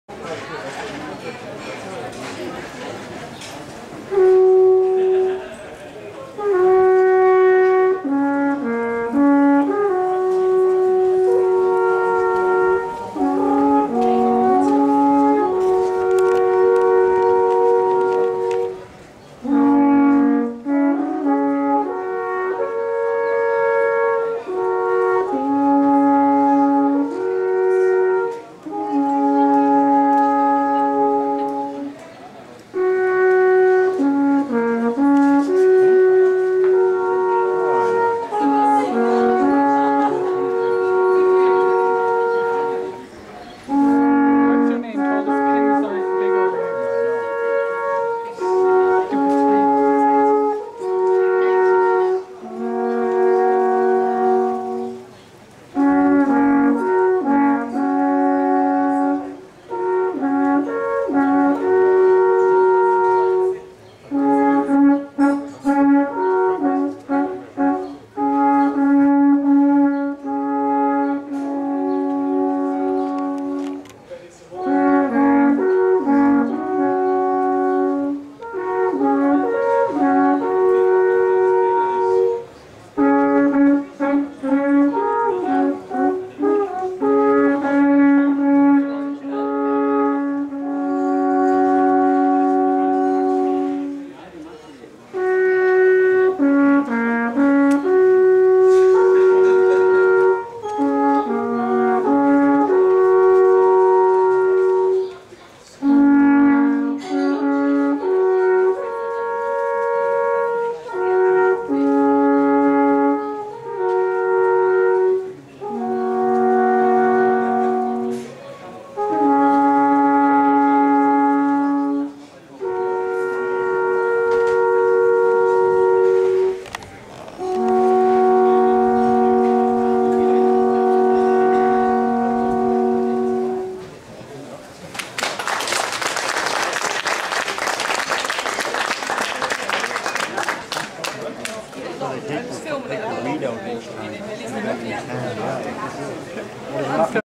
Alpine Horns @ Zermatt
We were treated to a small group of Swiss performers when we visited Zermatt. We listened to them sing, play spoons, a coin in a bowl, but this was the ultimate...the Alpine horns!